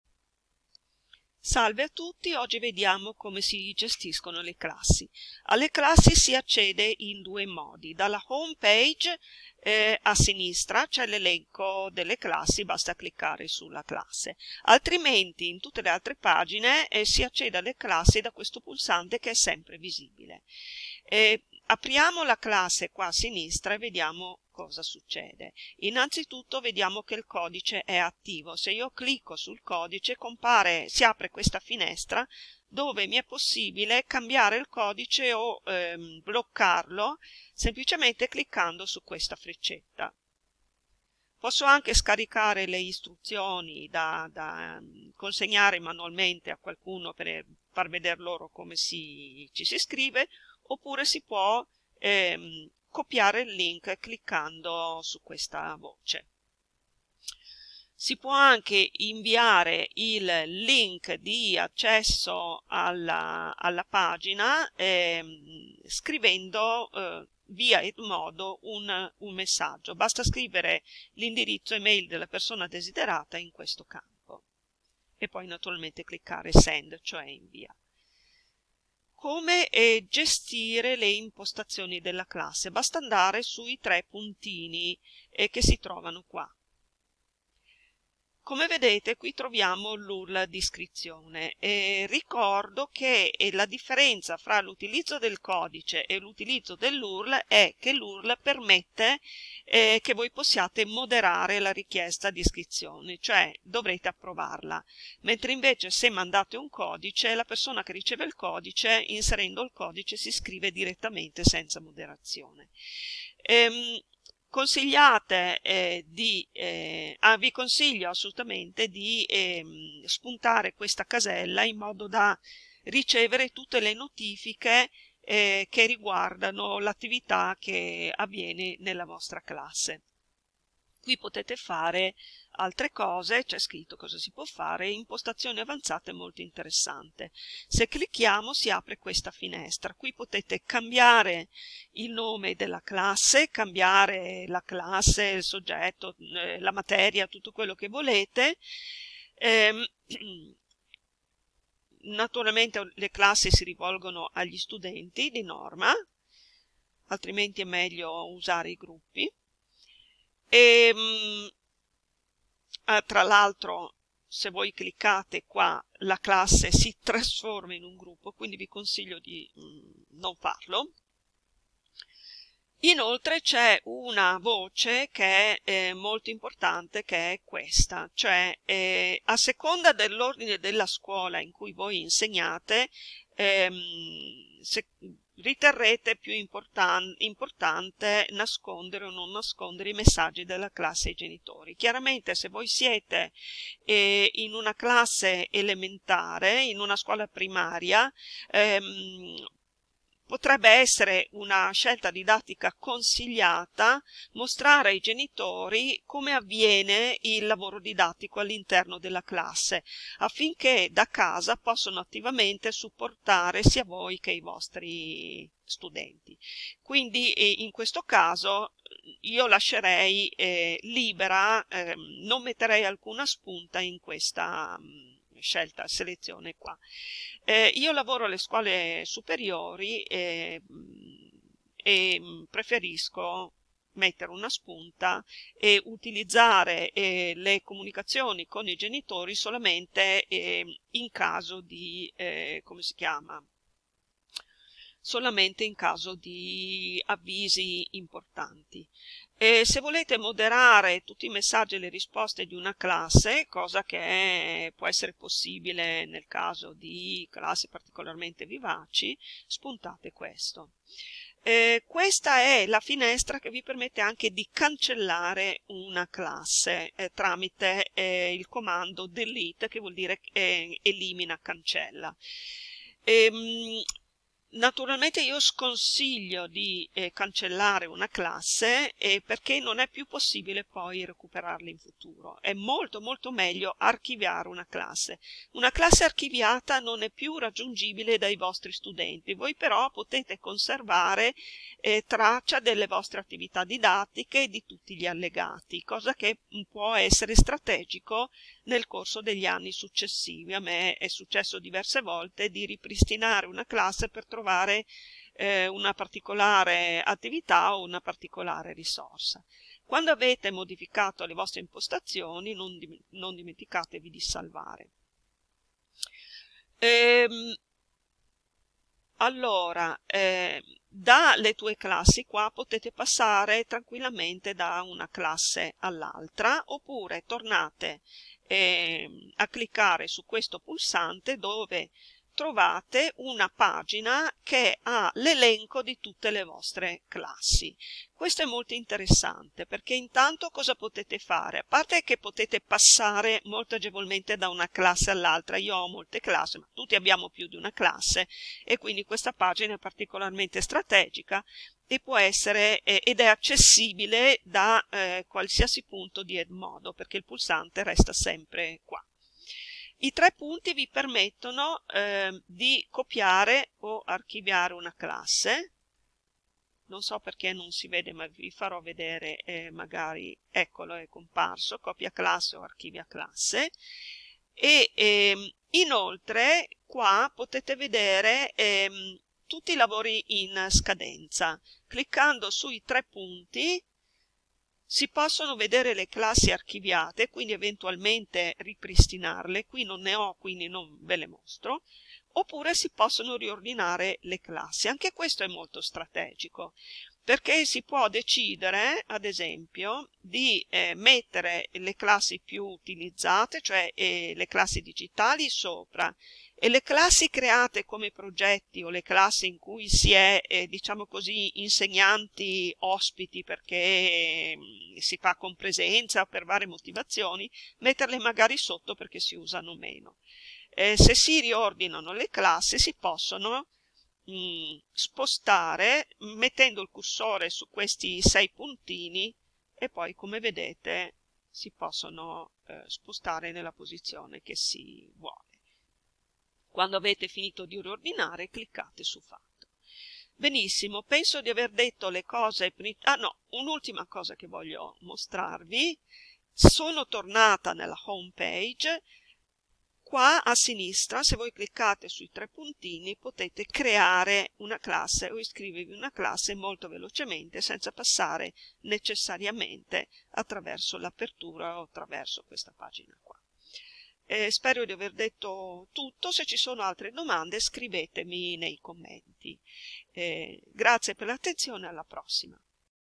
Edmodo 2019 La gestione delle classi
Il videotutorial fa riferimento alla versione di Edmodo completamente rinnovata nell'estate del 2019